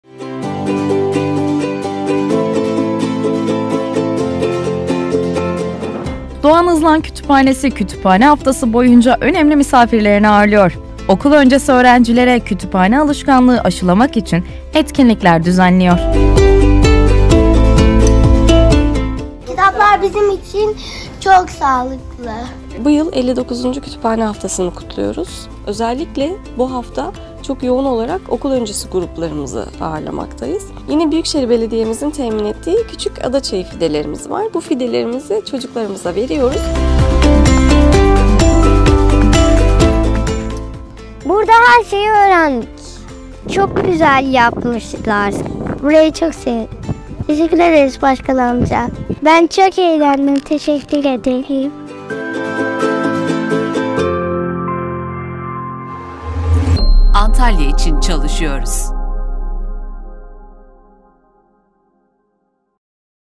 Kütüphane Haftası miniklerle kutlandı
Doğan Hızlan Kütüphanesi, Kütüphane Haftası boyunca okul öncesi öğrencileri ağırladı. Hafta boyunca gelen öğrencilere kütüphane ile ilgili bilgiler verilirken eğitimin ardından ABB tarafından adaçayı fidesi hediye edildi.

Oynatma Listeleri;